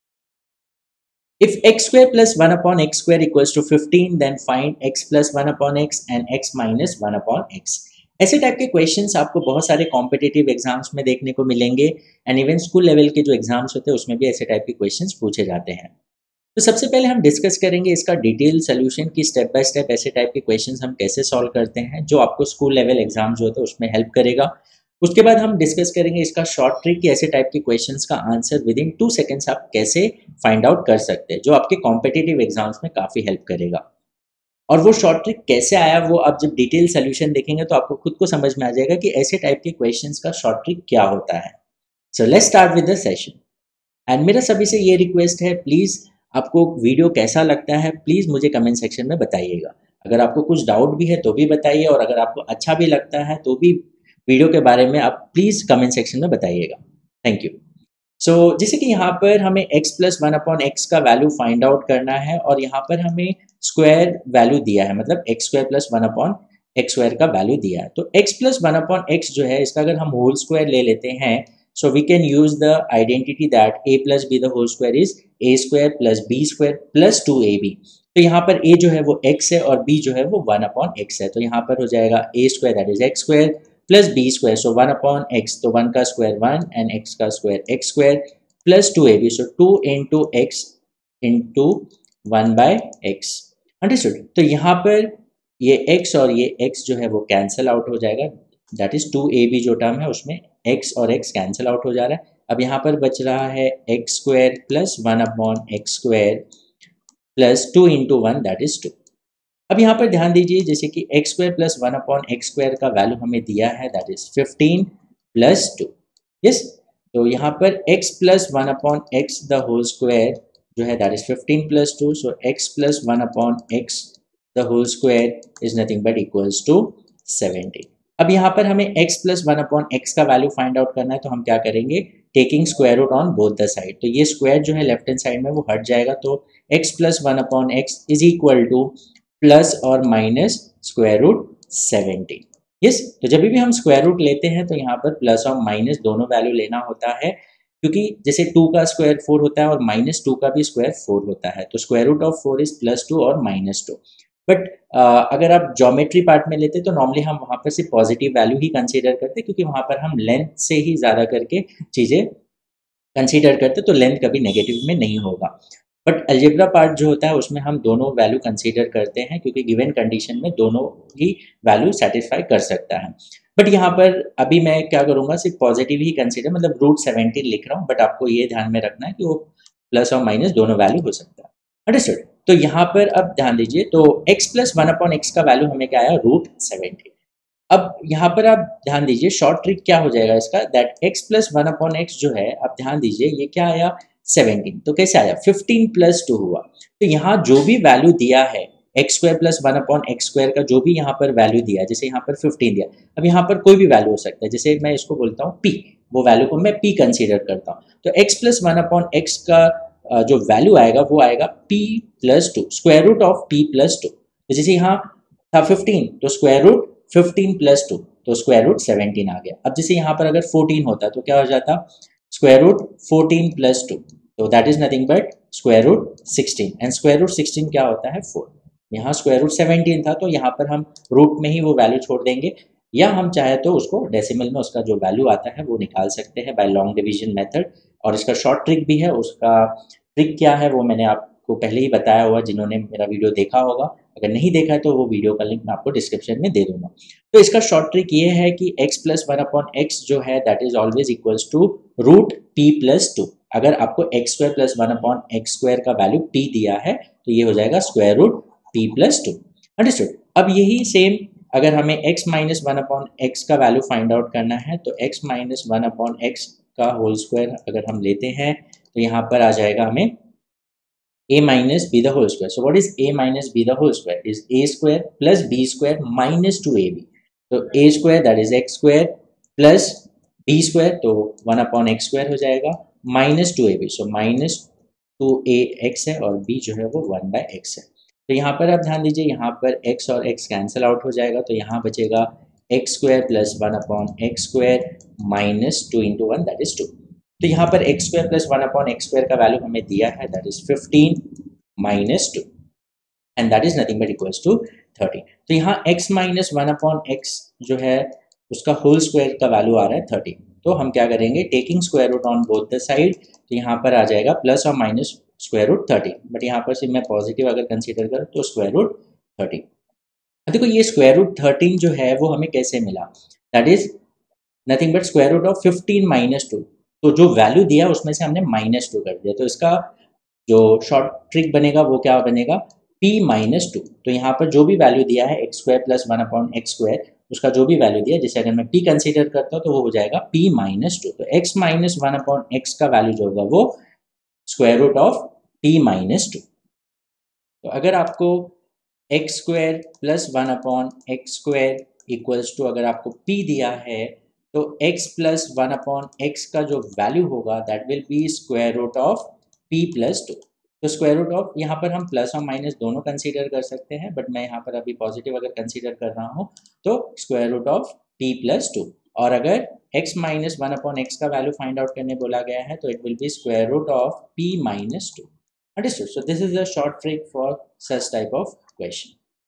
if x2 + 1/x2 = 5 then x + 1/x & x - 1/x | x + 1/x tricks | math tricks calculation tricks in maths
maths shortcuts for fast calculation